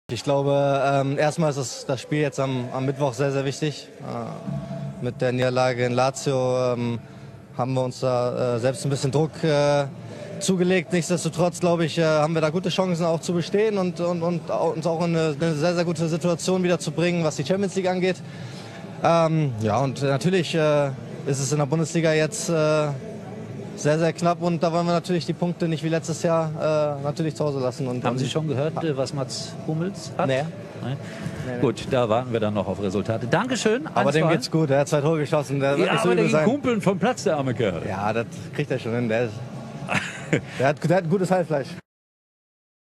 Julian Brandt besorgt über Verletzung von Mats Hummels
Dortmund vs. Arminia 2:0

Tore
Mats Hummels 53' und 71'